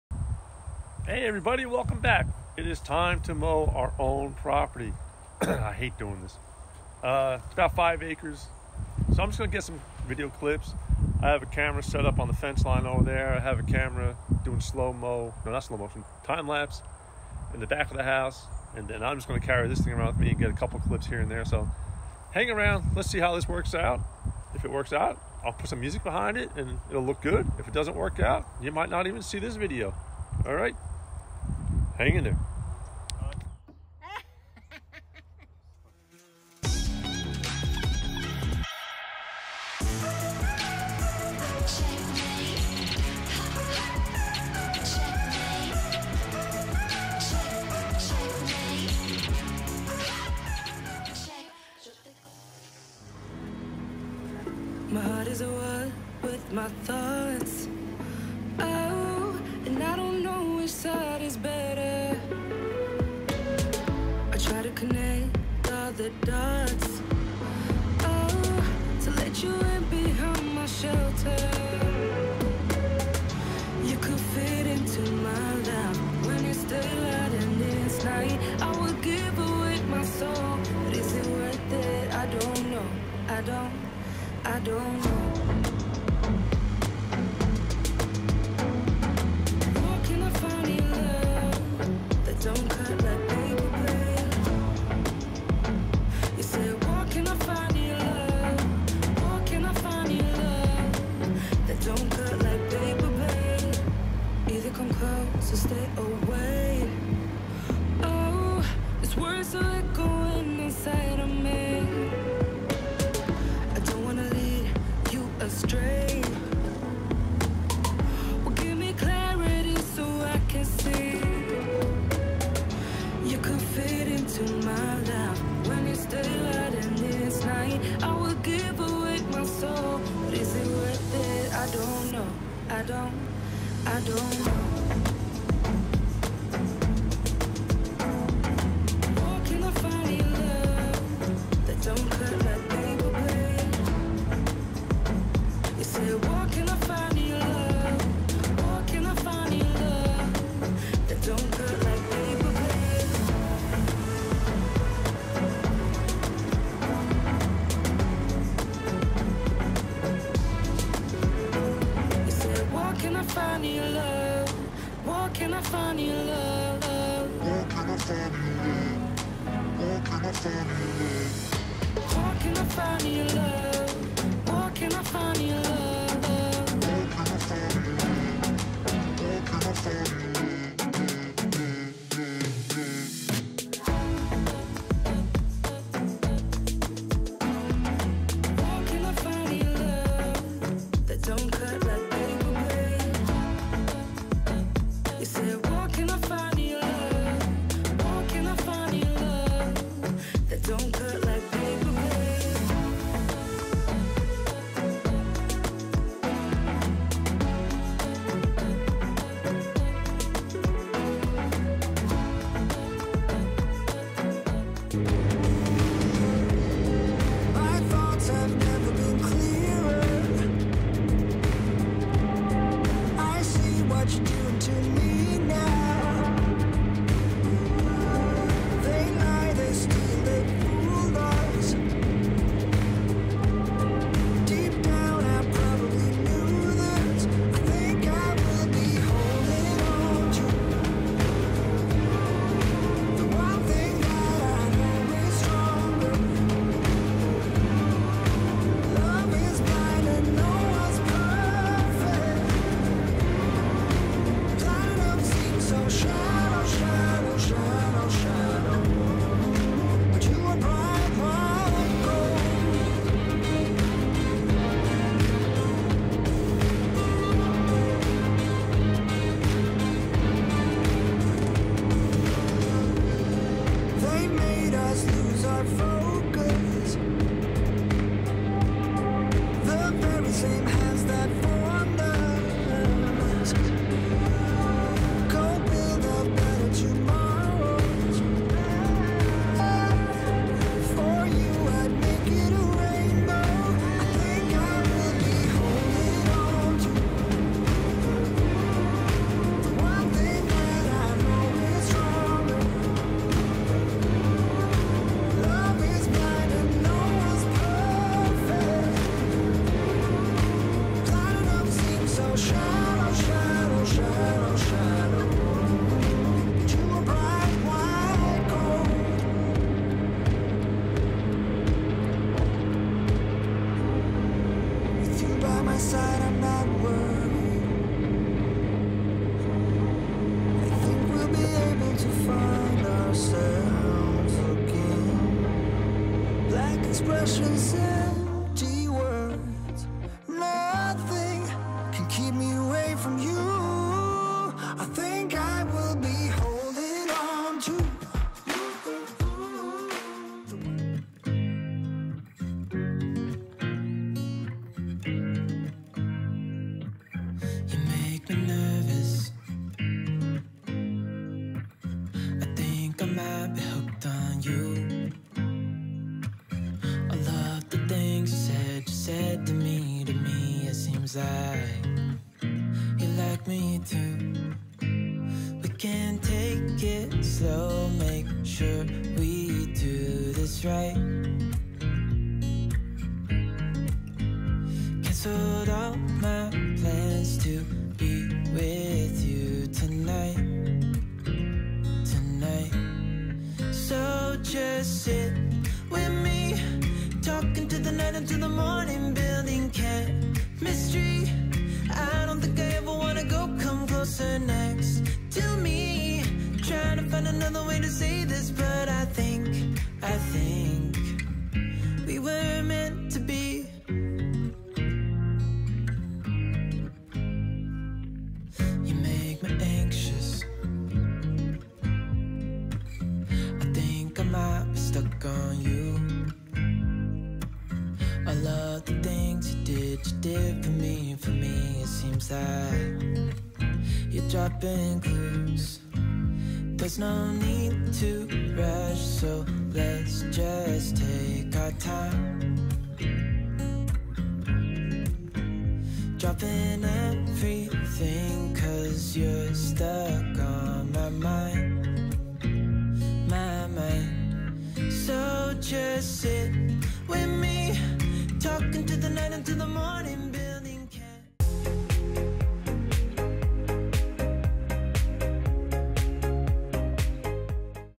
Mowing our own property, time lapse and some music.
starting to take advantage of multiple cameras, I decided to film myself mowing our own propery and see what routines are needed for multi camera filming